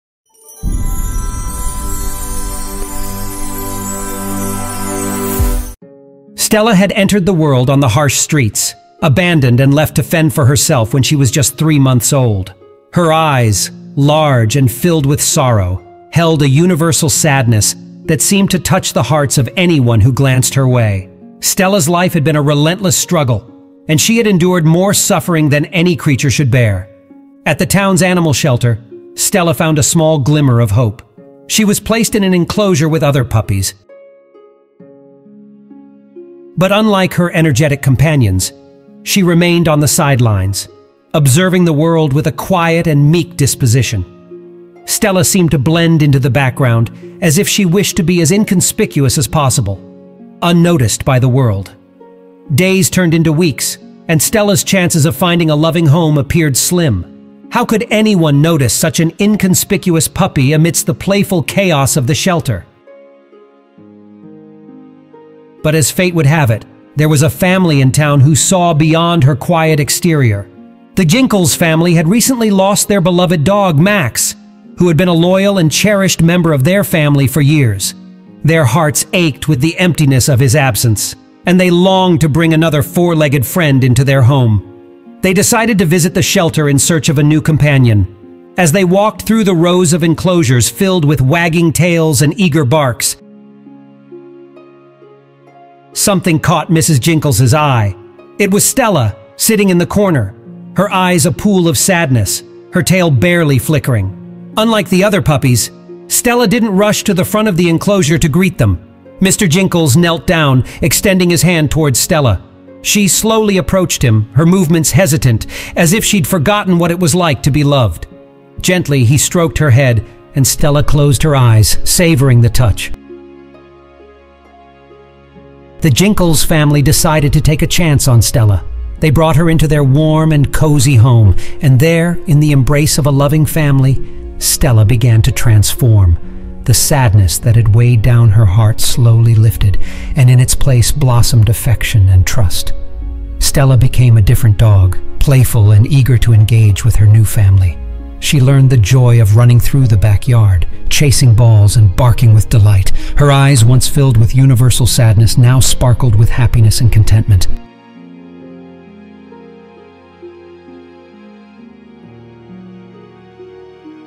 The Puppy Had Seen a Lot of Things and Looked At the World with a Faded Gaze
Her eyes, large and filled with sorrow, held a universal sadness that seemed to touch the hearts of anyone who glanced her way.